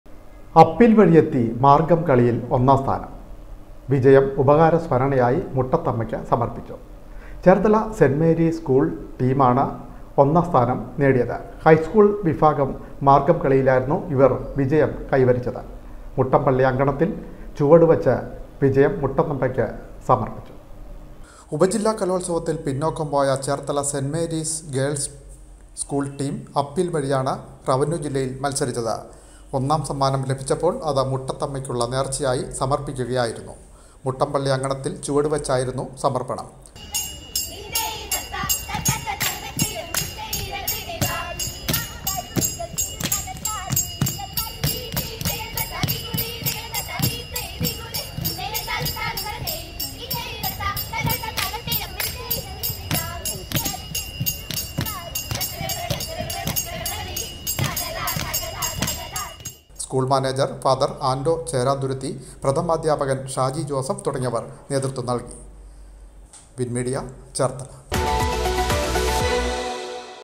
അപ്പീൽ വഴിയെത്തി മാർഗ്ഗംകളിയിൽ ഒന്നാം സ്ഥാനം..
വിജയം ഉപകാരസ്മരണയായിമുട്ടത്തമ്മയ്ക്ക്
സമർപ്പിച്ചു..